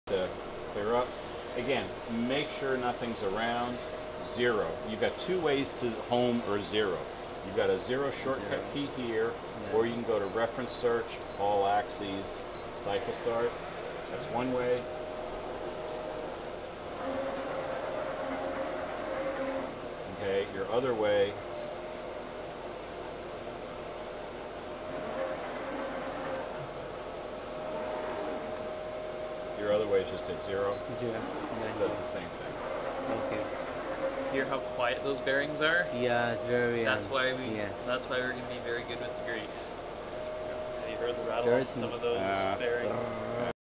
Home your FMT Patriot Machine
Ralph Pohl describing how to home the FMT Patriot on an installation for a customer.